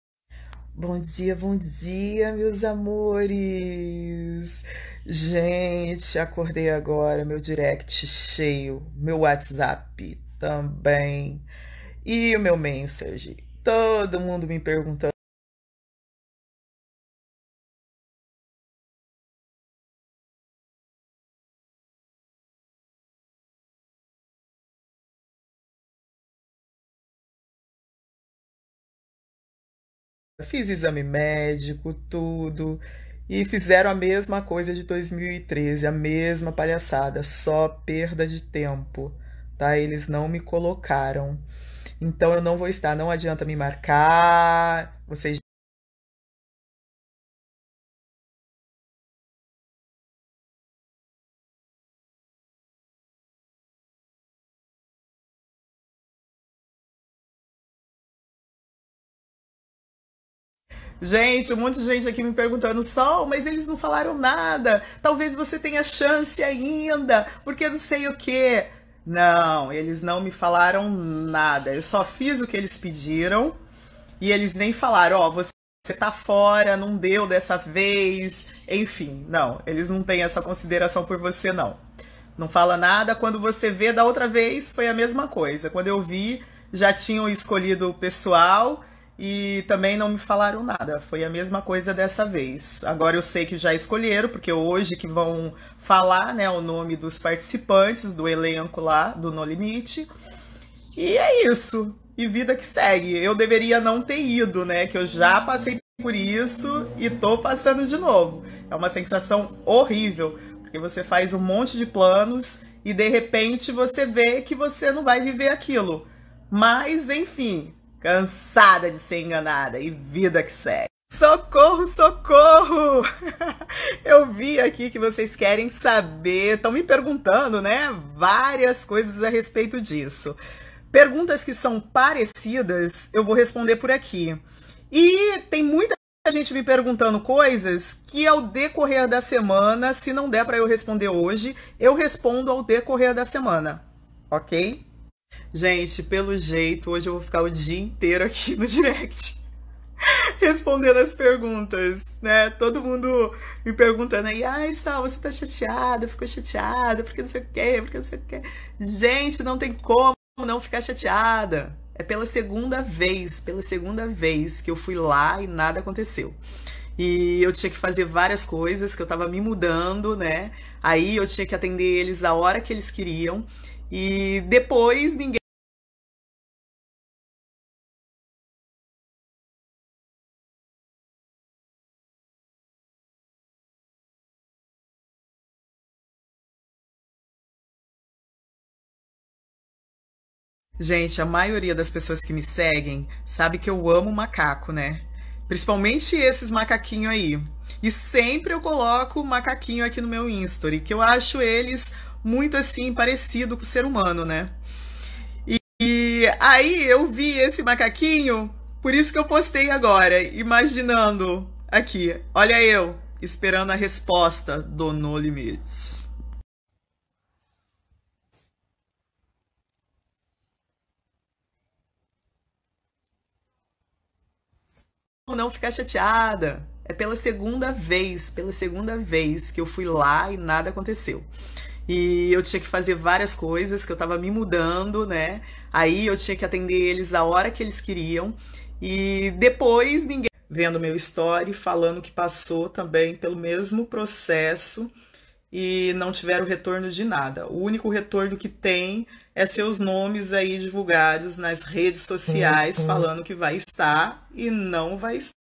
Solange (BBB4) revela que foi barrada do No Limite e expõe podres
[ AVISO! ] VÍDEO COM PROBLEMAS DE ÁUDIO